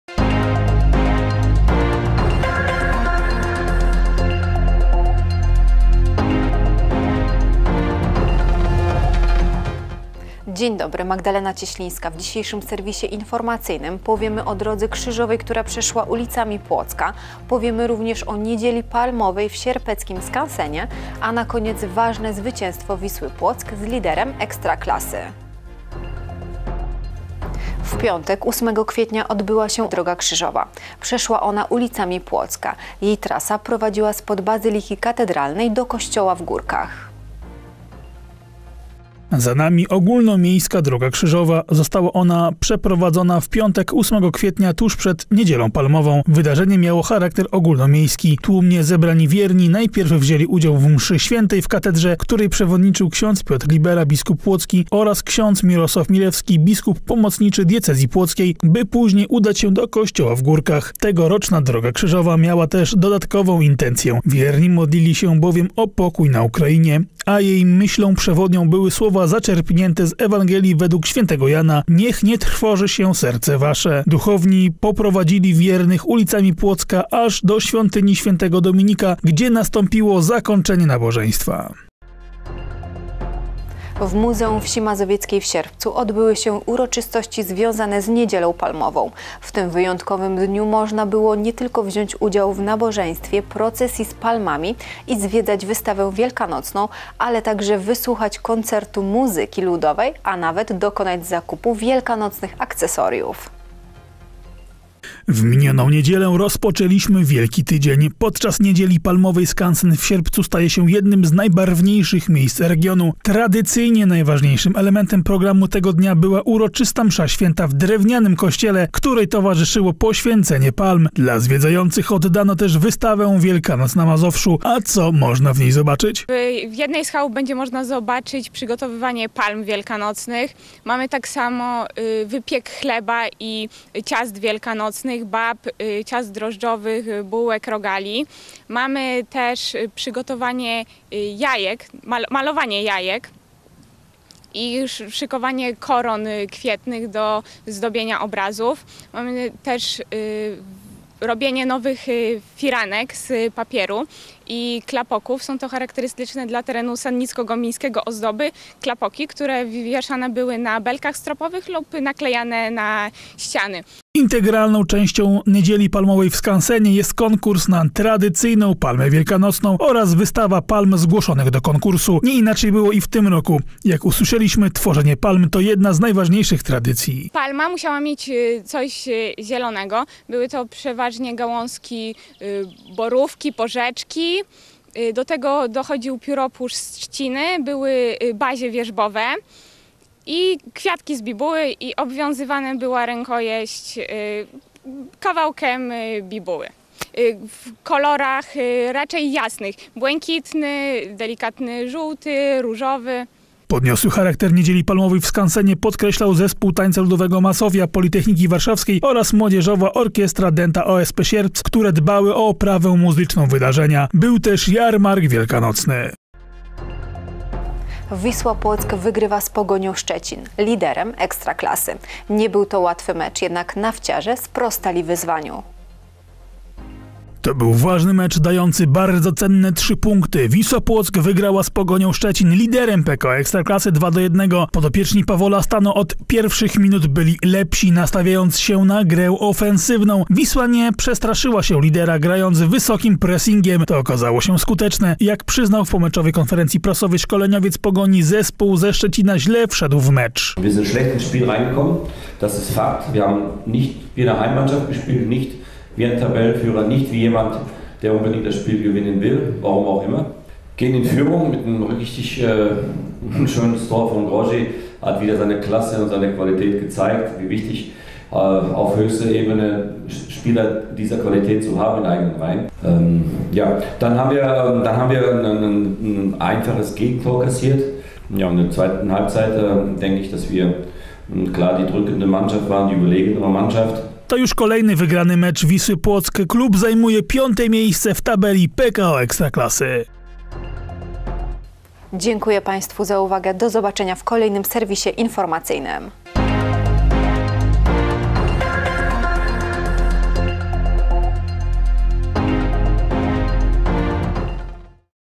Informacje 11.04.2022
W dzisiejszym wydaniu "Informacji PTV":

- ogólnomiejska Droga Krzyżowa ulicami Płocka,
- Niedziela Palmowa w sierpeckim skansenie,
- a na koniec ważne zwycięstwo Wisły Płock z liderem Ekstraklasy.